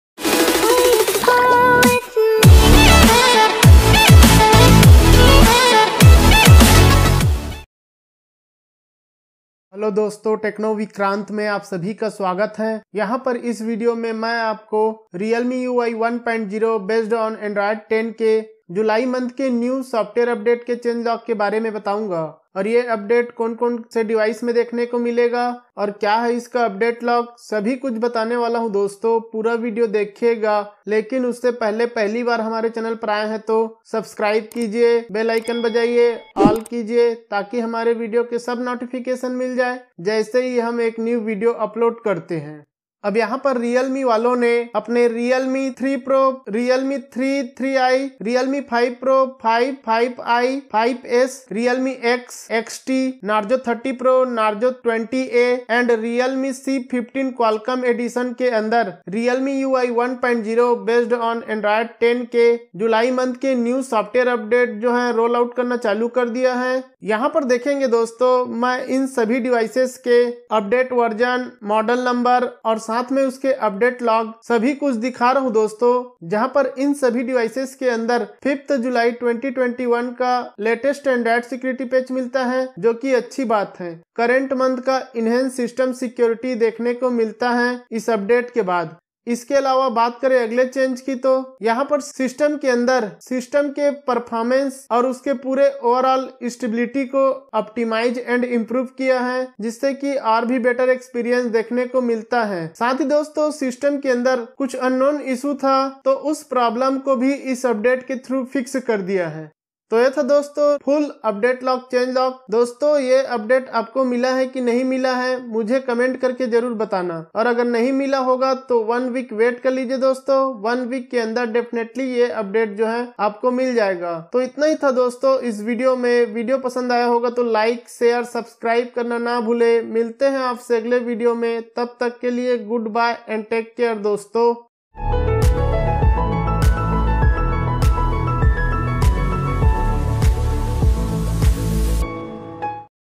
Realme July New Software Update | Realme 3 Pro,3,3i, 5 Pro,5,5i,5s, X, XT, Narzo 30 Pro, C15 QE 😱⚡
Dosto is video me maine aap logo ko Realme ke Realme UI 1.0 ke July month ke Stable Software Update ke bare me bataya hu. July Stable Update me Realme UI 1.0 or Android 10 ke New Features add hua or kuch known bugs fixed kiye hai. Umeed hai aapko video pasand aayega